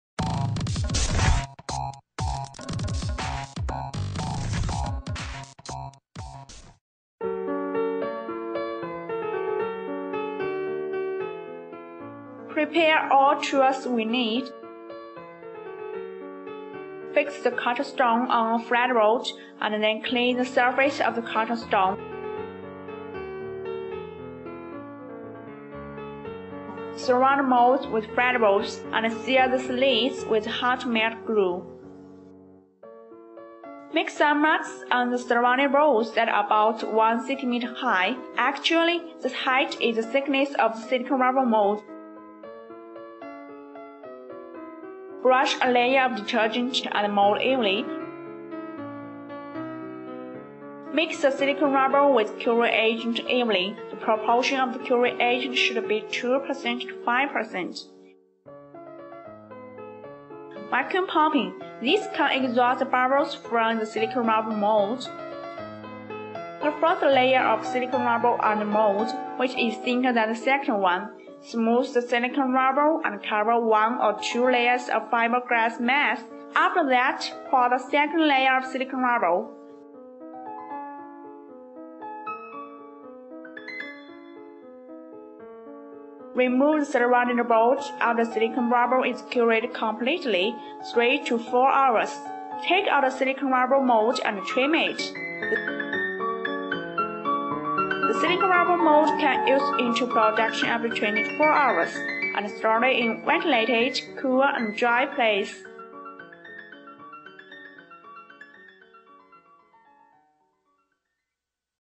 How Its Made - Culture Stone Mold by Silicone Rubber
The culture stone is made by silicone rubber, Mould making silicon rubber is suitable for industries such as toys,
gifts, decoration, plate engrave, human replica, art crafts, resin crafts, poly resin crafts, unsaturated resin crafts,
candle crafts, plastic crafts, stationeries, plaster crafts, Buddhist statue crafts and so on.

The moldmaking RTV silicone rubber can be classified according to its curing method as either a condensation-cure silicone
 or an addition-cure silicone and they are widely used to make replicas with a wide variety of different materials including
 polyester and epoxy resins, urethane foam, wax, gypsum, concrete and so on.

Jenova Silicone is specially focus on RTV-2 MOLDMAKING SILICONE RUBBER since 1994.